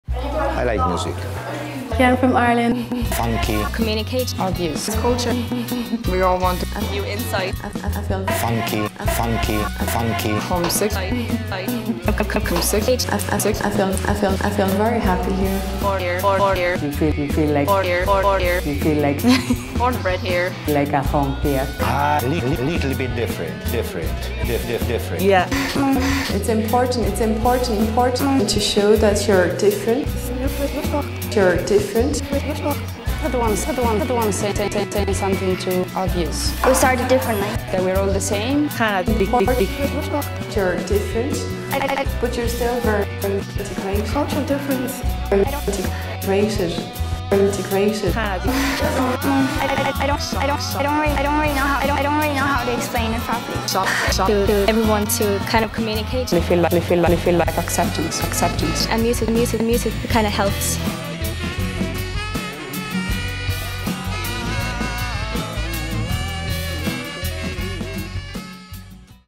Mixed & Mumbled
'Mixed and Mumbled' is the latest film, photography and music project created by the Carlow Community Group, 'Pure Thinking'. Produced by Connie Byrne Hyland and the CBH Design team, featuring the work of photographer Agata Stoinska, musician Eddie Sheehan and film maker Marc-Ivan O'Gorman.

This clip features the subject of the project, that is, the various members of the local community who do not originate from Ireland but currently contribute to its daily life. The accompanying music, by THE SPINNING BOY, features Paul Galbally on Bass, Mervyn Sayers on Saxophone and Dhruv Sangari on vocals. The clip was shot and edited by Cleverality productions.

'Mixed & Mumbled' launches in VISUAL Centre for Contemporary Art, Old Dublin Road, Carlow on Saturday 18th September at 7.30pm